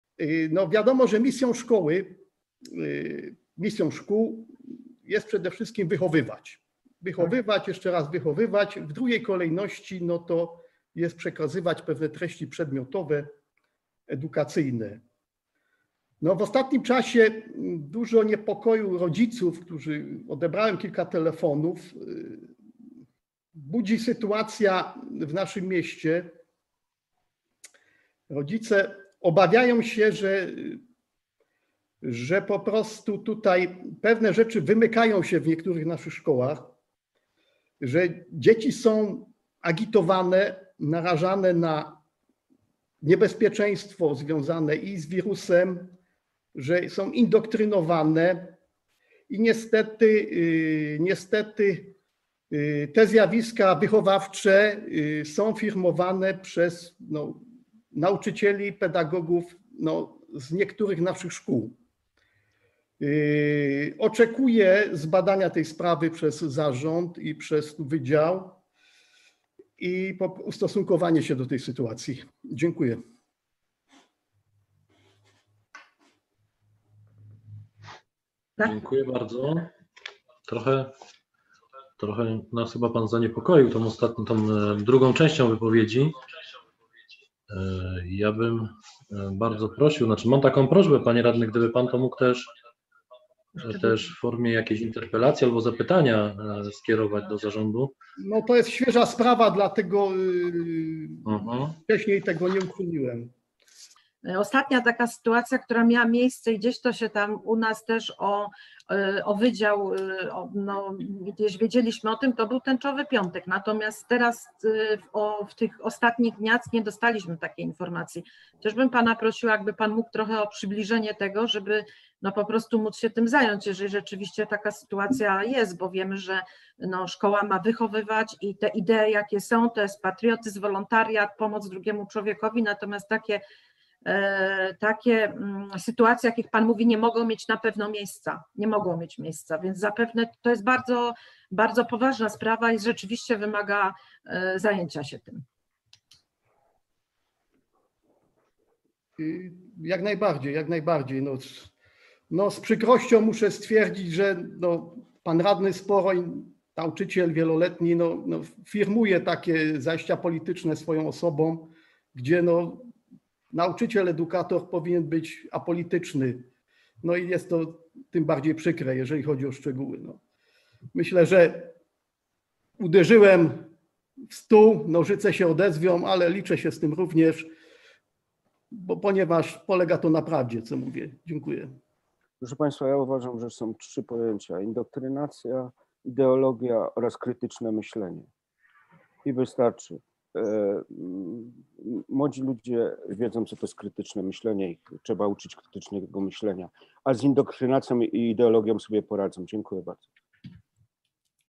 Radny PiS: w naszych szkołach dochodzi do agitacji i indoktrynacji - Tarnowskie Góry
Protesty, które w Tarnowskich Górach koncentrują się przed biurem poselskim Barbary Dziuk, odbiły się echem na wtorkowej sesji rady powiatu tarnogórskiego.